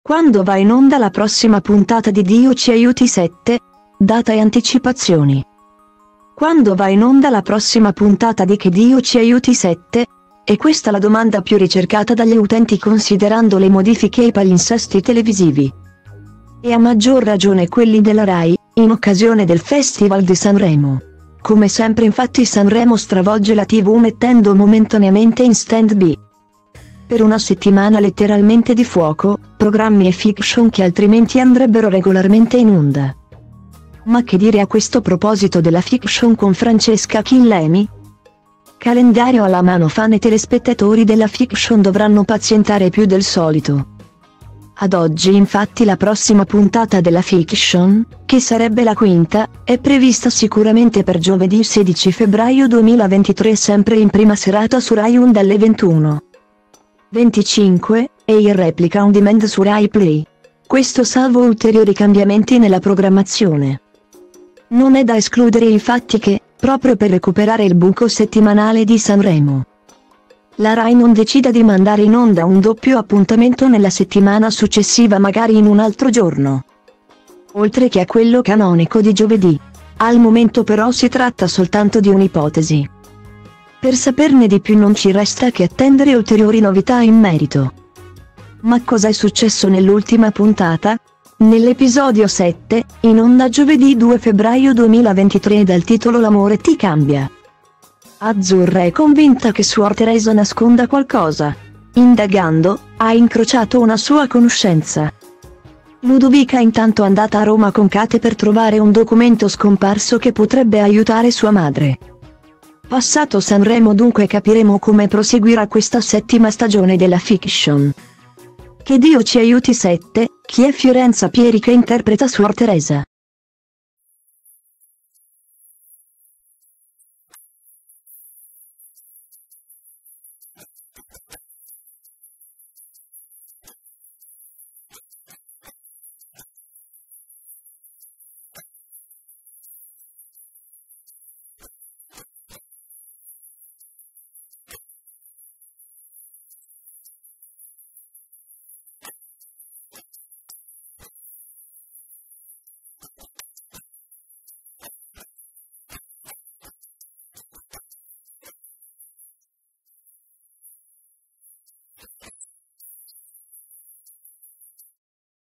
Quando va in onda la prossima puntata di Dio ci Aiuti 7? Data e anticipazioni
Quando va in onda la prossima puntata di Che Dio ci Aiuti 7? E’ questa la domanda più ricercata dagli utenti considerando le modifiche ai palinsesti televisivi,e a maggior ragione quelli della Rai, in occasione del Festival di Sanremo. Come sempre infatti Sanremo stravolge la tv mettendo momentaneamente in stand-by,per una settimana letteralmente di fuoco, programmi e fiction che altrimenti andrebbero regolarmente in onda.